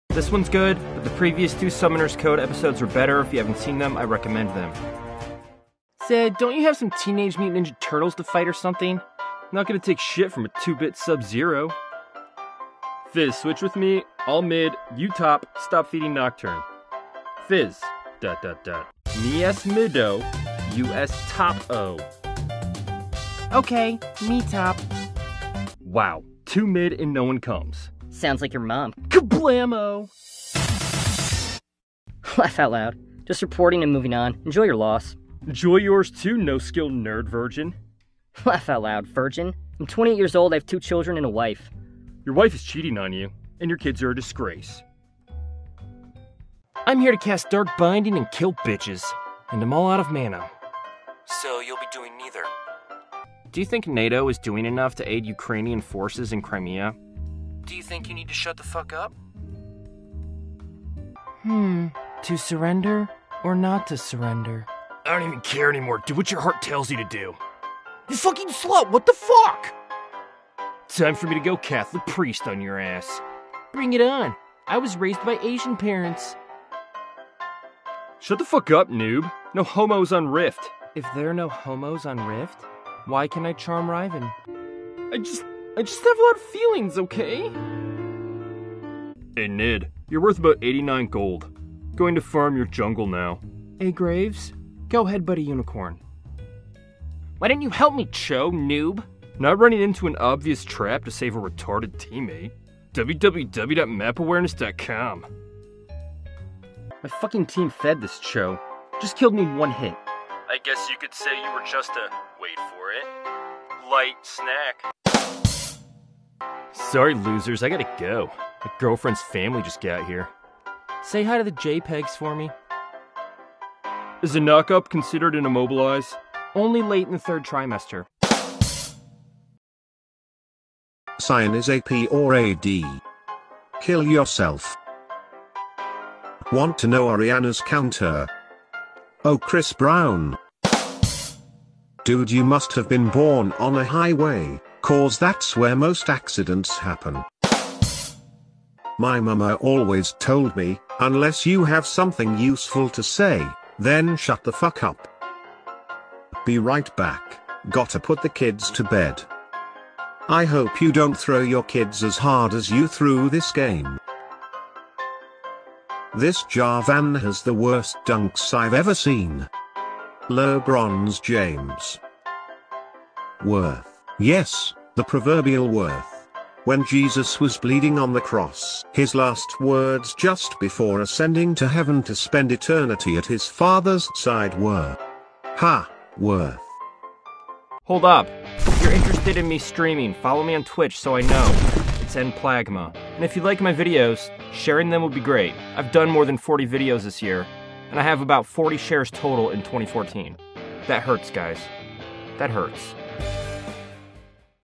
FUNNIEST SUMMONER'S CODE 3 (#37)
The rest of the funny submissions to Summoner's Code that I'm going to do. I'll be going back to using the material that I discovered, myself, and know to be authentic. Or maybe I'll stop making these